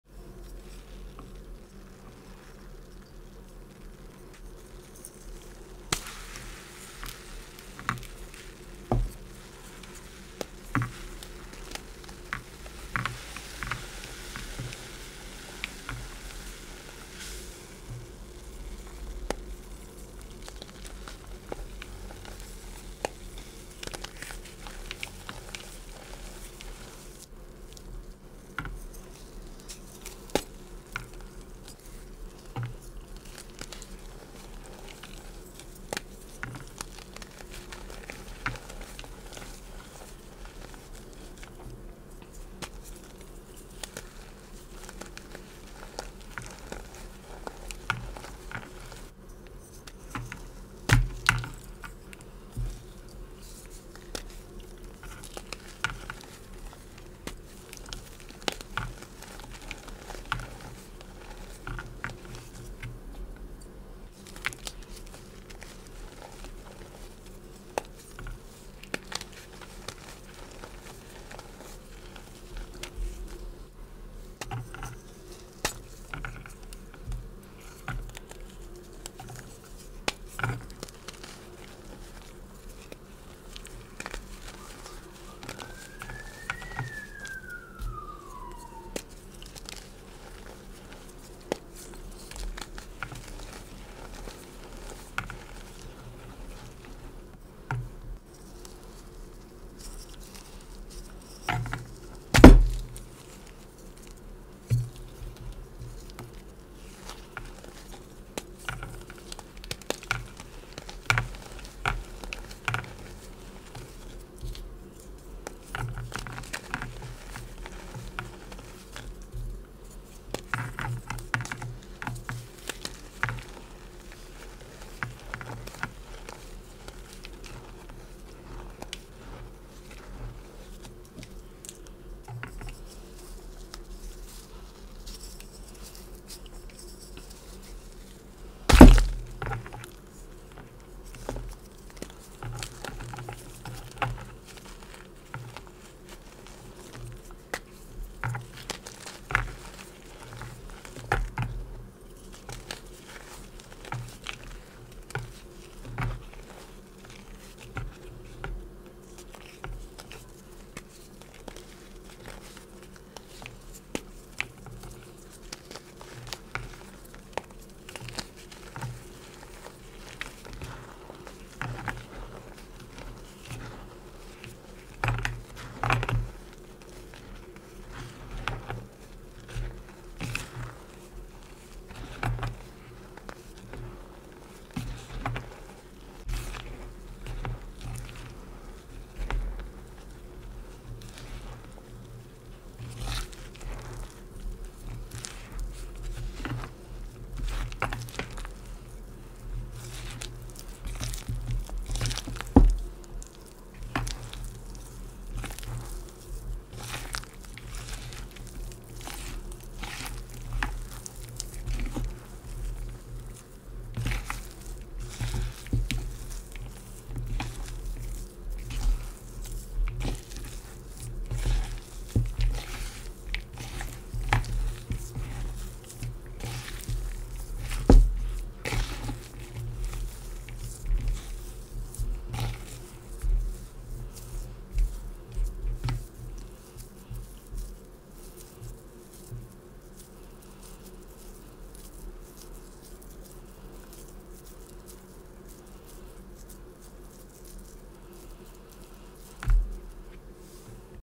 Thank you 1K on YT, TT, Ig
Thank you for over 1K on Ig, TT and YT. I am grateful for all the love and support. Please keep blessing me. I love you all. 🫶🏽🫶🏽❤️
Crunchy gymchalk reform balls water crush.